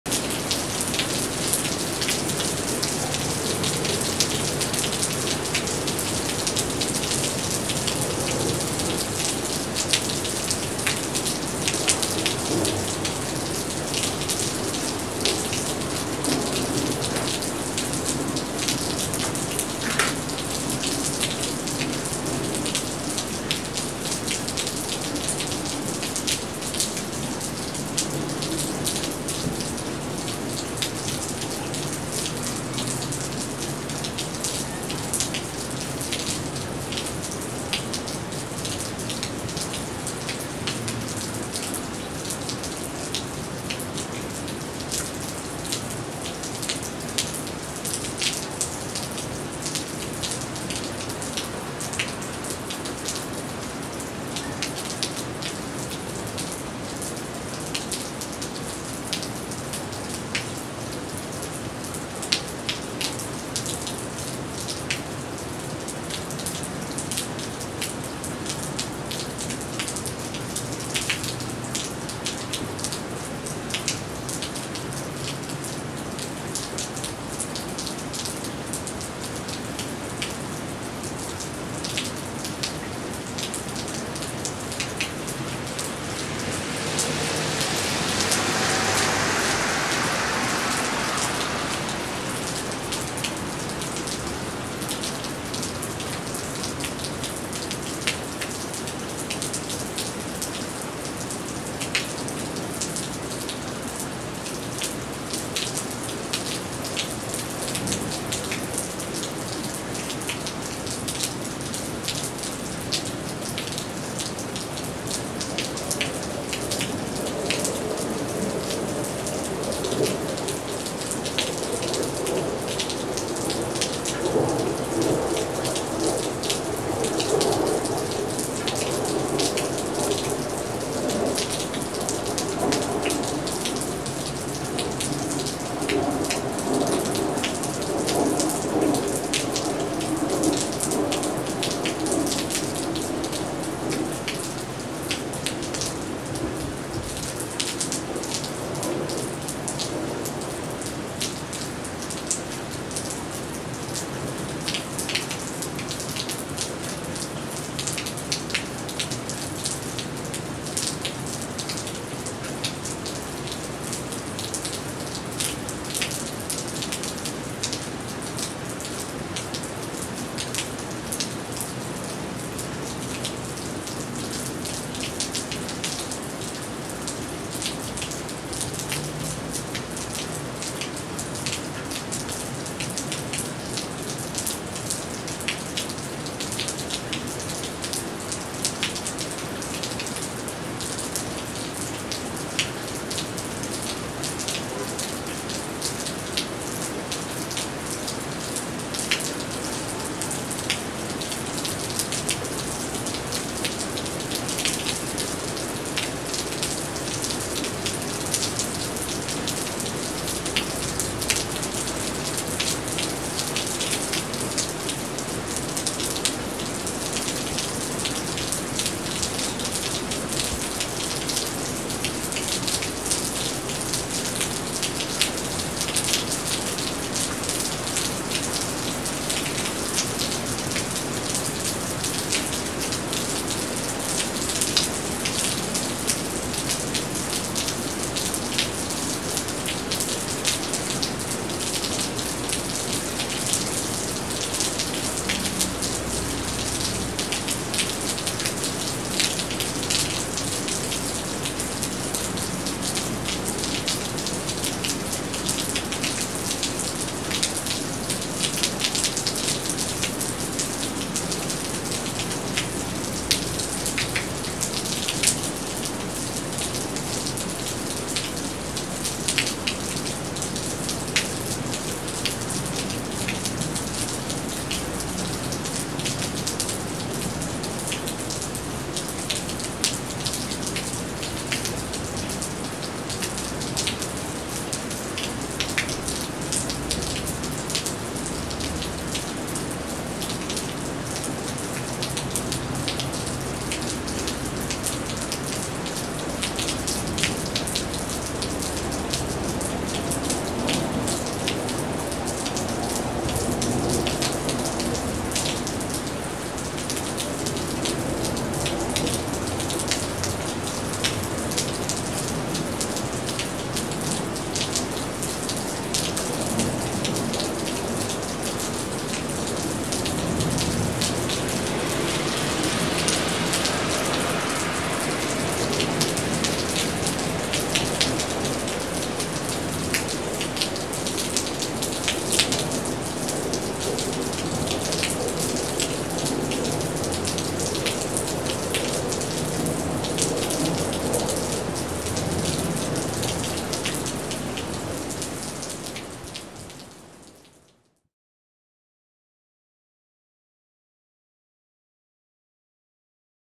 Gentle Rain Sounds
A rainstorm in San Diego, CA brought gentle relaxing rain showers.